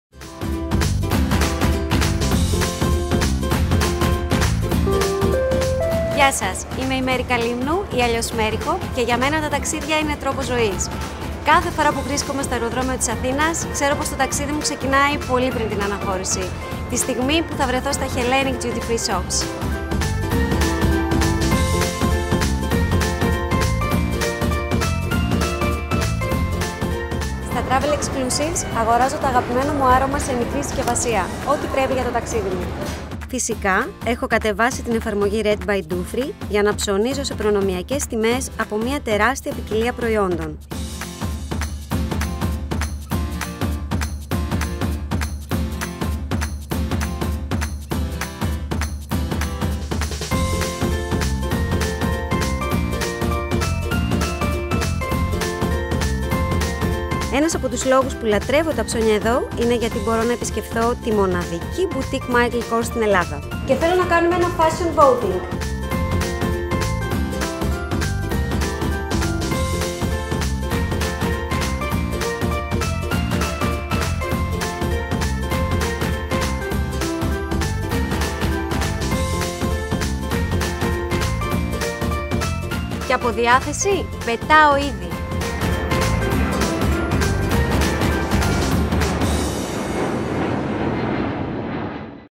Explore Hellenic Duty Free Shops with Mary Kalymnou
Learn how to shop smart at Hellenic Duty Free Shops, from an expert!
The awarded Travel Blogger Mary Kalymnou proves that stylish trips always start with a visit at the Hellenic Duty Free Shops!